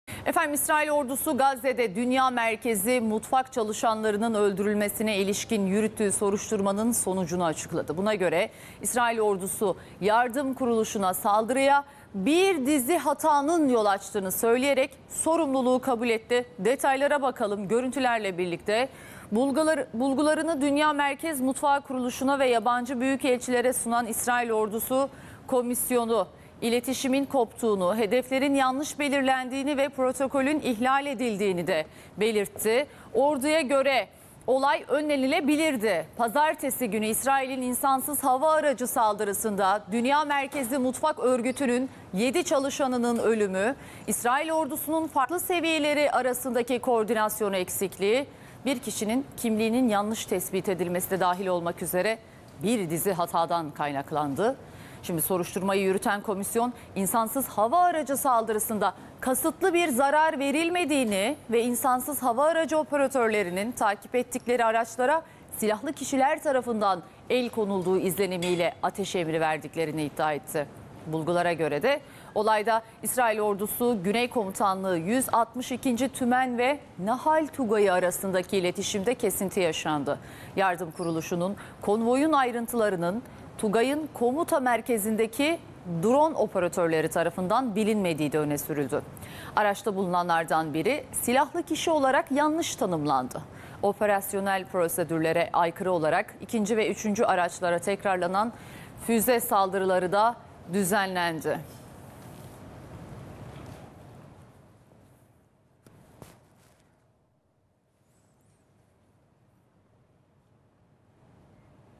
SON DAKİKA! 🚨 | Yardım Kuruluşuna Saldırı Soruşturması Sonuçlandı! İsrail Sorumluluğu Kabul Etti
İsrail Savunma Kuvvetleri, geçtiğimiz günlerde Deir el-Balah'ta “World Central Kitchen (WCK)” adlı insani yardım kuruluşu çalışanlarının bulunduğu araçları hedef aldı. Saldırıda yardım kuruluşunun 7 görevlisi hayatını kaybetti. İsrail yaşananların ardından özür diledi.